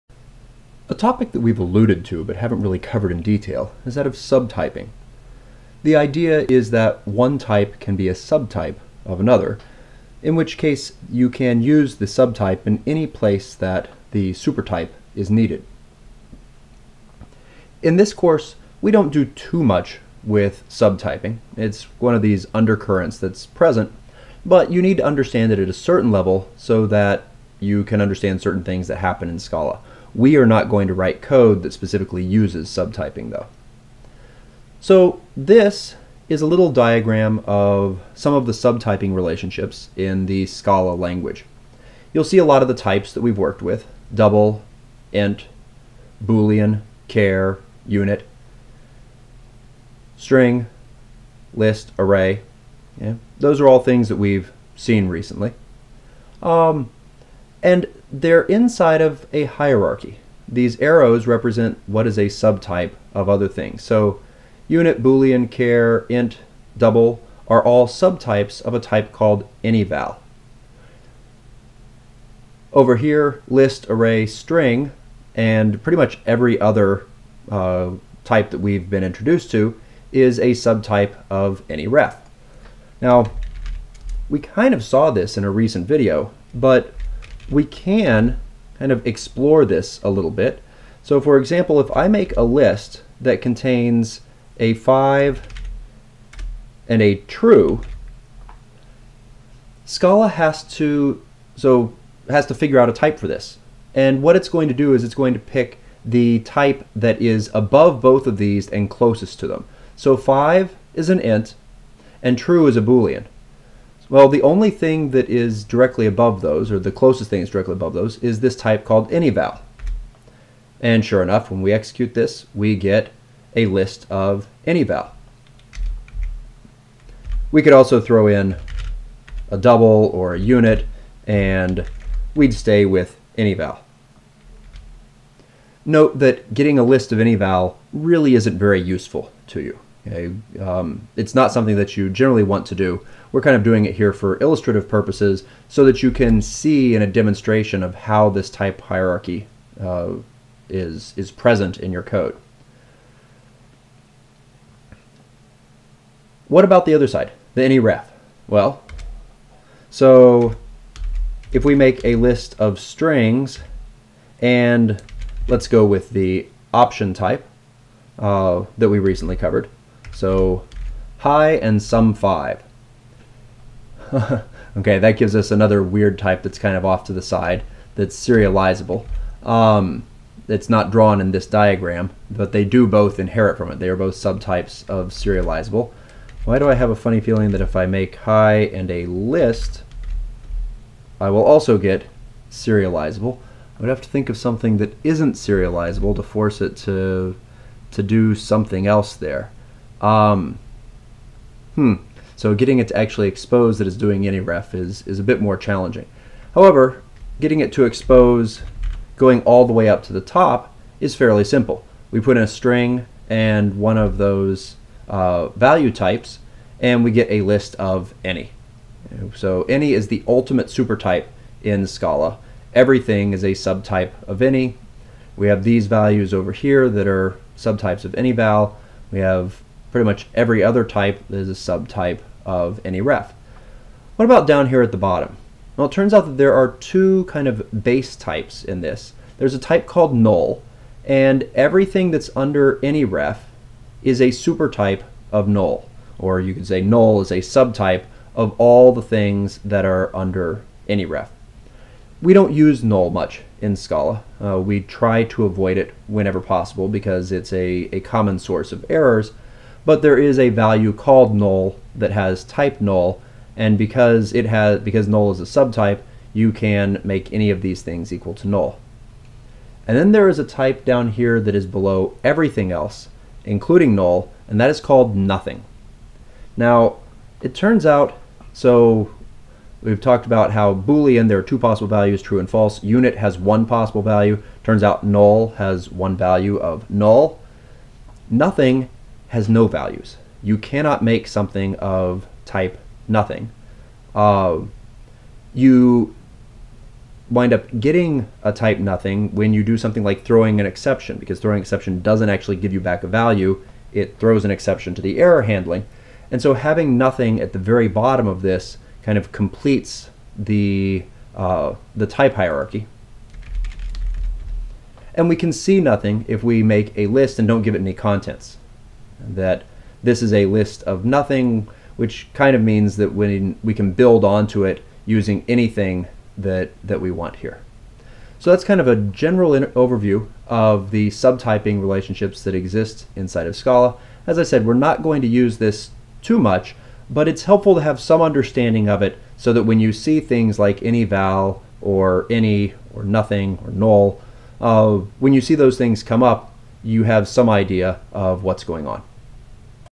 Subtyping (in Scala)
This video briefly covers the concept of subtyping and the type hierarchy in the Scala language.